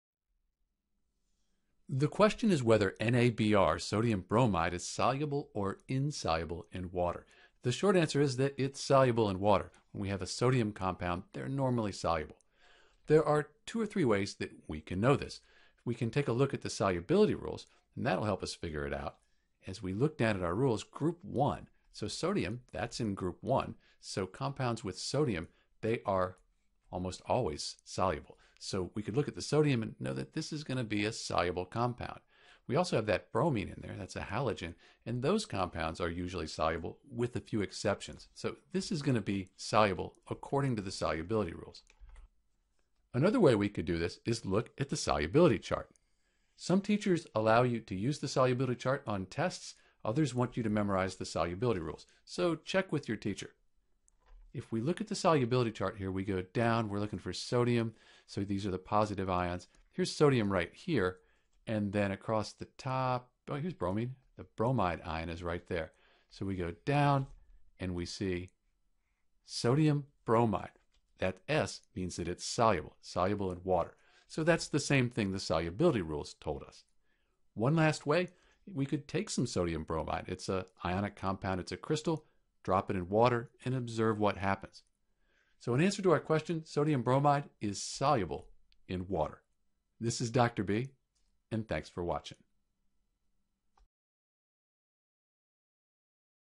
Is NaBr Soluble or Insoluble in Water?
Is NaBr (Sodium bromide) soluble or insoluble in water? The answer is that it is soluble in water. It is an ionic compound which readily dissociates into its ions in water.

We can also look a Solubility Table On the solubility table we can see that NaBr (Sodium bromide ) is soluble in water.

Solubility Resources:

We could also just get some NaBr and see if it dissolves in water, that would also tell us if it is soluble or insoluble.

While we could look at the solubility constant, called the Ksp, for NaBr it usually isn’t used as NaBr is a highly soluble compound. KsP is more appropriate for slightly soluble and insoluble compounds.